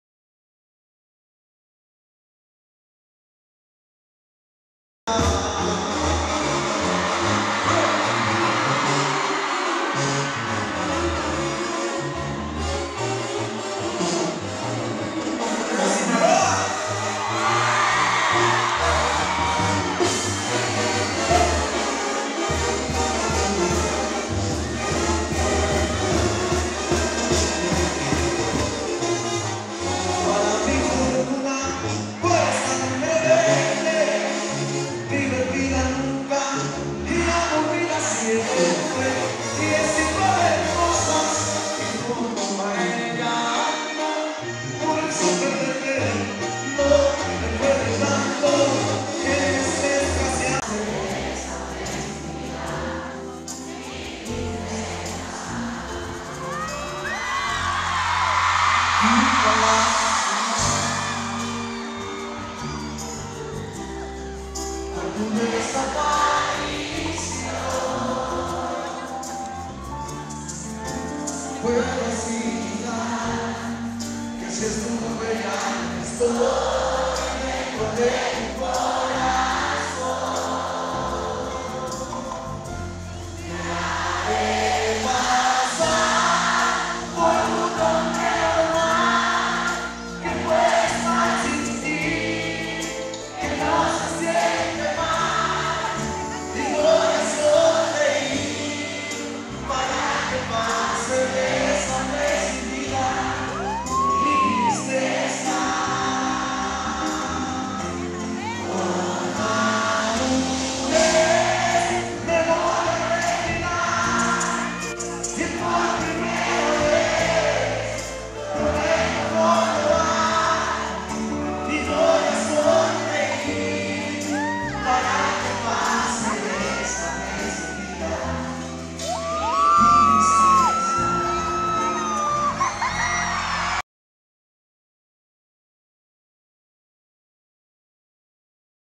Espinoza Paz @ Playas de Tijuana [HD][2]
Concierto de Espinoza Paz en La Monumental en Playas de Tijuana, B.C. Mexico.  22 de Mayo del 2012.

Espinoza Paz concert at La Monumental in Playas de Tijuana, B.C. Mexico.  May 22nd of 2012.